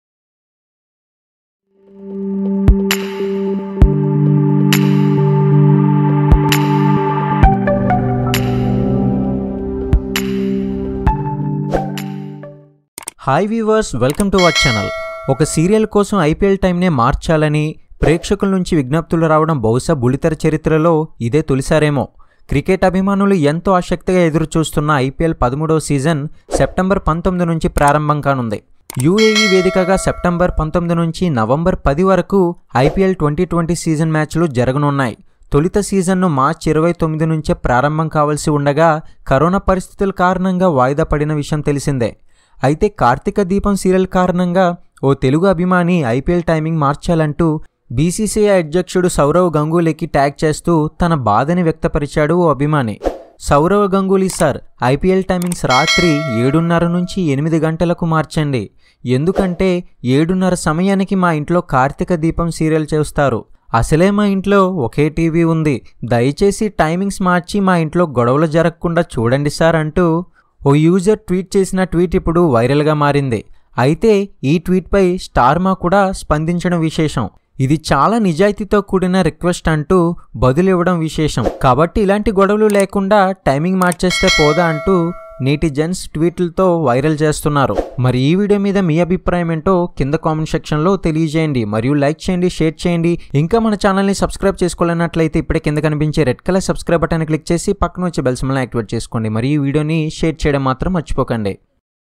సామాన్య యూసర్ కి బలే స్పందించారు | Karthika Deepam Serial Challenging Tweet | Premi Viswanath
సామాన్య యూసర్ కి బలే స్పందించారు | Karthika Deepam Serial Challenging Video Tweet Update | Premi Viswanath